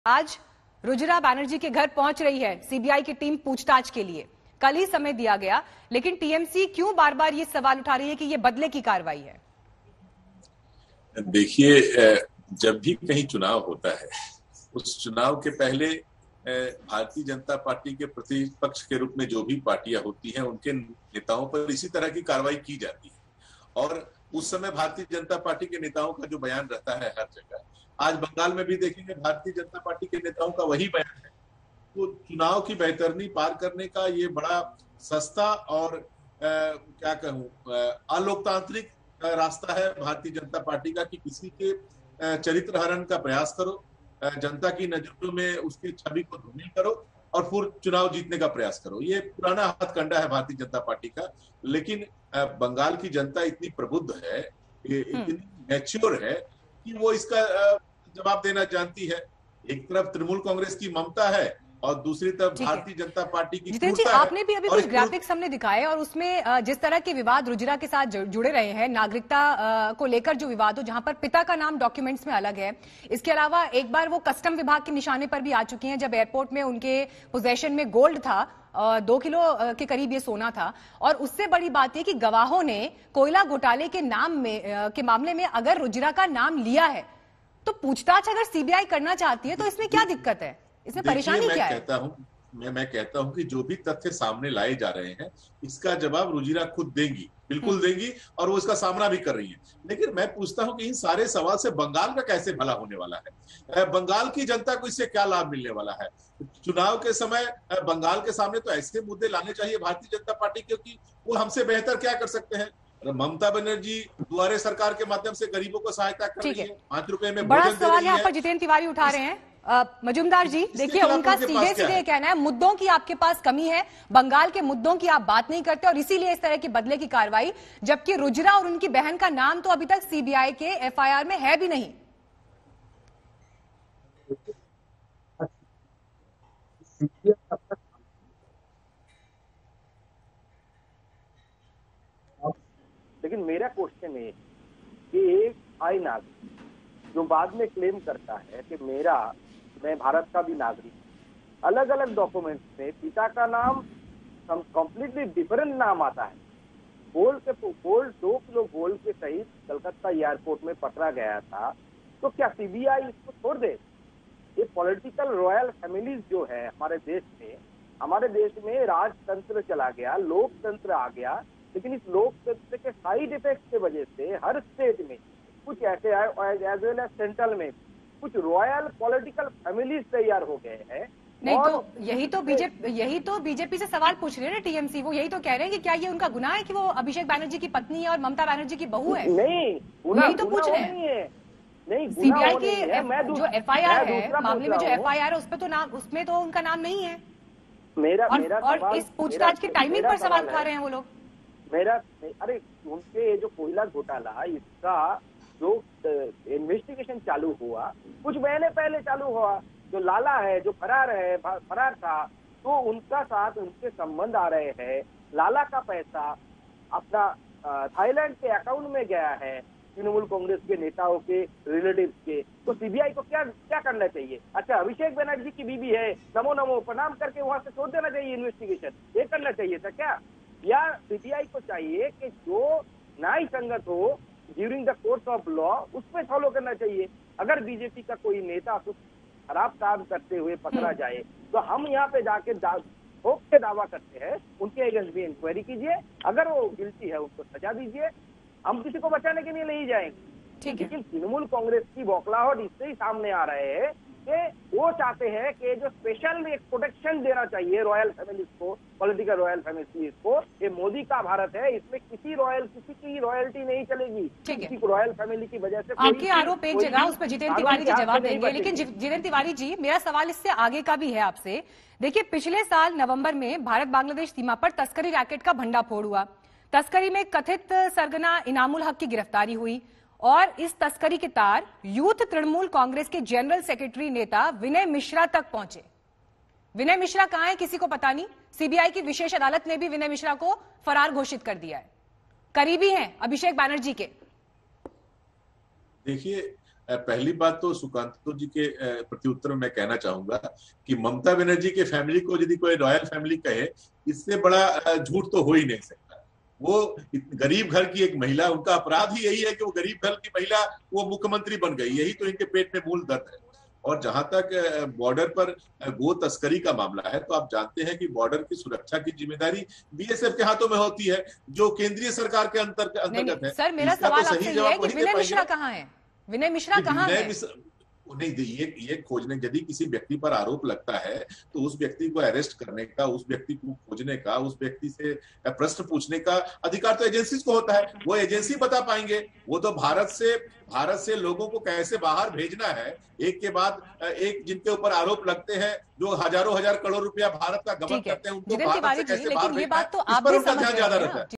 Bengal: TMC प्रवक्ता - जब भी कहीं चुनाव होता है, BJP प्रतिपक्ष पार्टियों पर ऐसे ही कार्यवाही करती है
Bengal: कोयला तस्करी केस में आज Mamata Banerjee की बहु यानिकि भतीजे Abhishek Banerjee की पत्नी Rujira Narula से आज CBI पूछताछ करेगी. सोमवार को Rujira से पूछताछ नहीं हो पाई थी. इसको लेकर जमकर सियासत हो रही है. तो वहीं आज तक के इस खास चर्चा में TMC प्रवक्ता Jitendra Tiwari ने BJP पर निशाना साधते हुए कहा- जब भी कही चुनाव होता है, उस चुनाव से पहले BJP के प्रतिपक्ष के रूप में जो भी पार्टियां होती है उनके नेताओं पर इसी तरह की कार्यवाही की जाती है. देखें वीडियो.